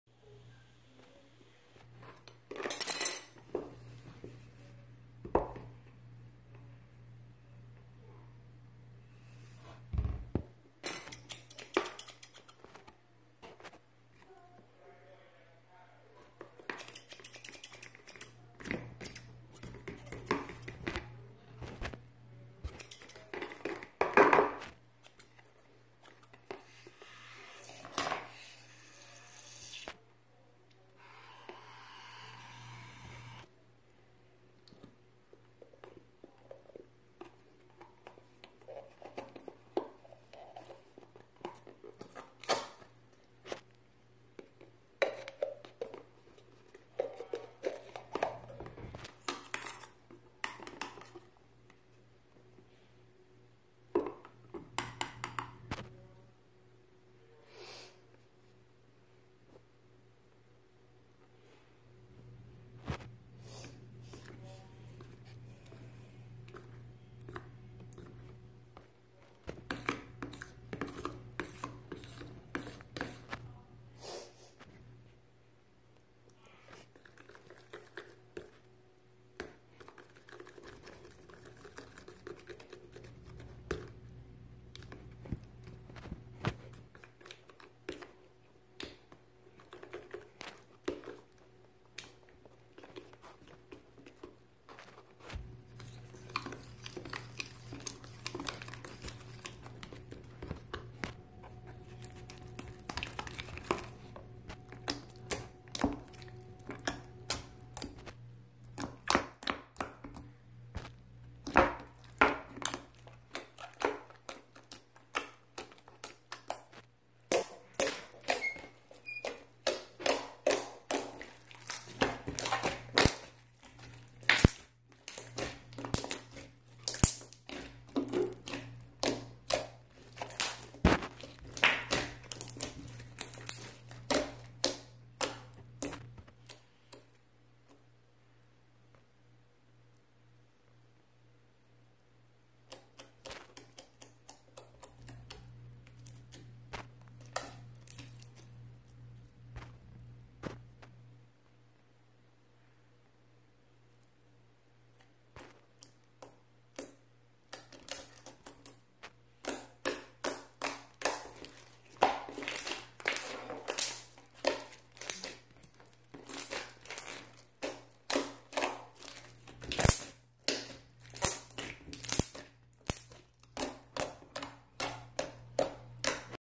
Water wiggle (holiday version)🧤
Making a clear slime with a ball filled with water and a charm

By the way the ball was really hard to open with the scissors (Dont take it the wrong way)